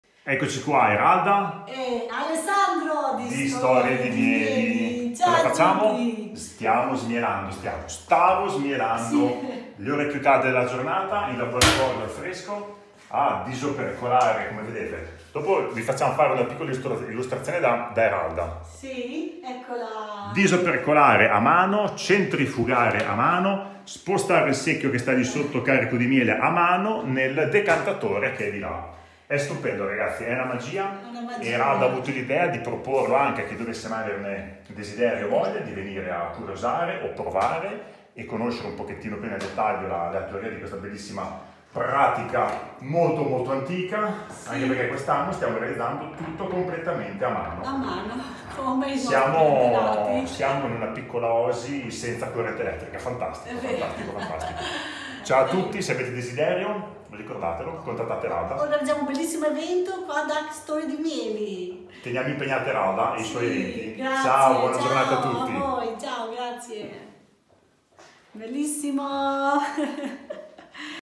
Smielatura
Che gioia amici la smielatura.. dopo tanta fatica nell'inseguire le fioriture, nel sostenere con amore pr le api e la natura arriva il momento magico, che ci emoziona ogni volta.. Il profumo inebriante del miele riempe la stanza del laboratorio, la disopercolatura a mano , la centrifugazione sono momenti meravigliosi per noi!!
Venite a trovarci, saremmo lieti di avvicinarvi a questo mondo meraviglioso con il sorriso e l'amore.

visita il nostro sito e ordina il tuo miele preferito